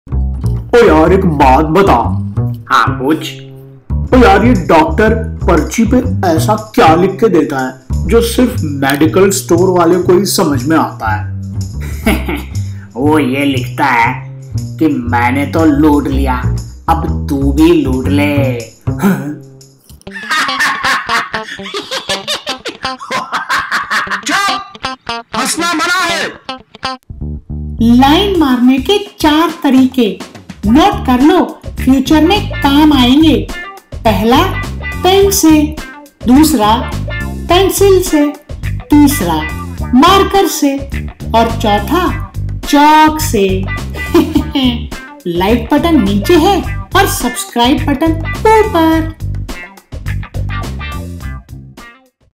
Jokes In Hindi - Hansna Mana Hai - Ep 09
संता : यार एक बात बता ?
बंता  : हाँ पूछ।
संता : यार ये डॉक्टर पर्ची में ऐसा क्या लिख के देता है, जो सिर्फ मेडिकल स्टोर वाले को ही समझ में आता है?
बंता : हे हे , वो ये लिखता है, की मैंने तो लूट लिया, अब तू भी लूट ले।

We give you the latest Animated joke in our web series 'Hansna Mana Hai'. Hope you are enjoying the jokes.